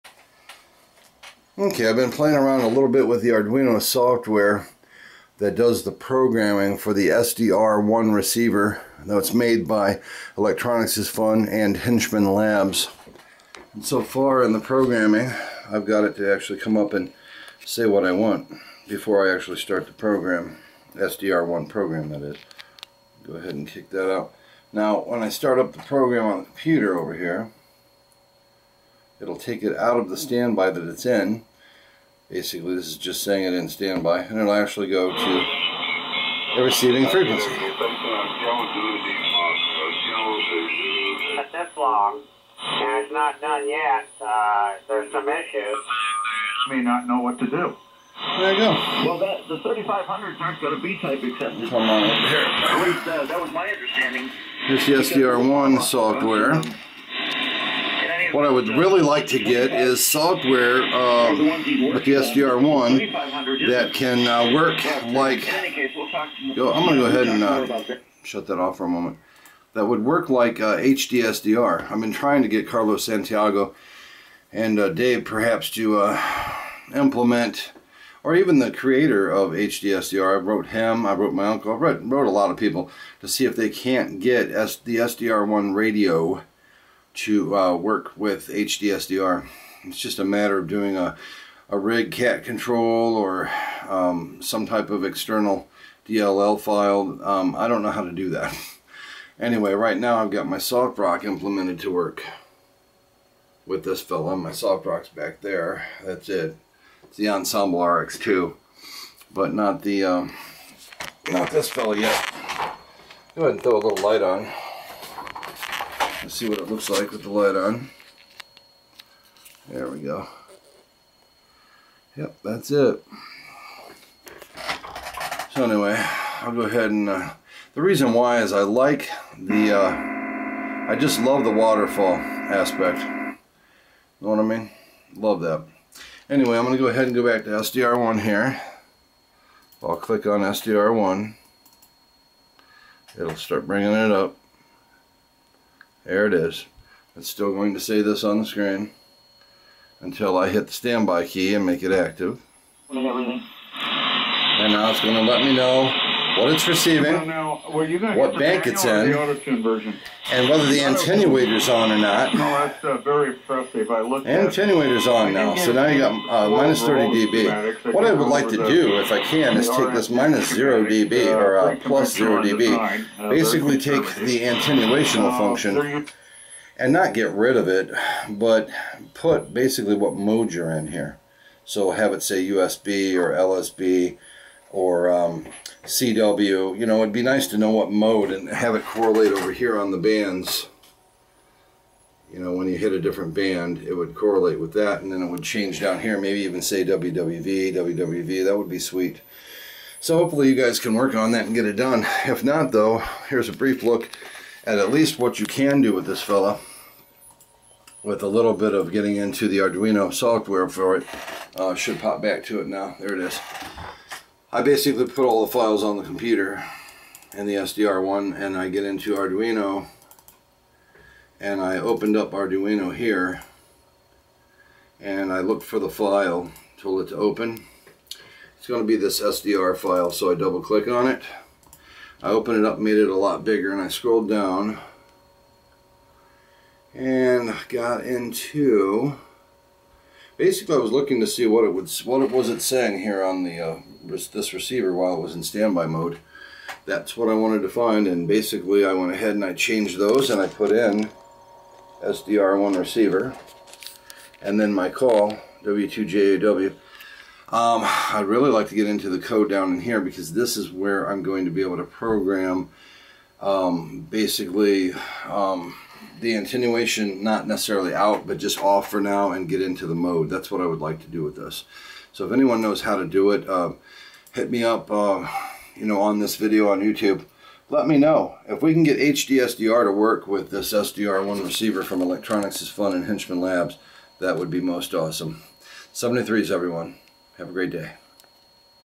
SDR-1 Receiver from Electronics is Fun and Henchman labs
This is an awesome and very beautiful radio, and I do love the SDR-1 Software.

It was a blast to put together and the instructions were excellent.

Guru even took an hour or so out of his day to talk to me on the phone and help me to better understand the SDR-1 and it's Arduino firmware.  He helped me so much that I'm able to change the LCD in standby mode to say whatever I want for it to say. (That's cool!!)

I would love however to be able to use the SDR-1 Receiver with my other favorite SDR program. HDSDR.

If someone can please make an EXTio_SDR-1.DLL file, that would be great as I have no idea how to compile the file to make the SDR-1 work with HDSDR!

or just make it so that the SDR-1 Radio can work using HDSDR by the Cat or Rig Control program.

I'm good with hardware, but not the software aspect.

Guru at Electronicsisfun.com and Henchmen labs, even my uncle.   Hopefully someone can help.

Thanks everyone and thanks to Electronicsisfun and Henchman labs for a great SDR radio!!

John... W2JAW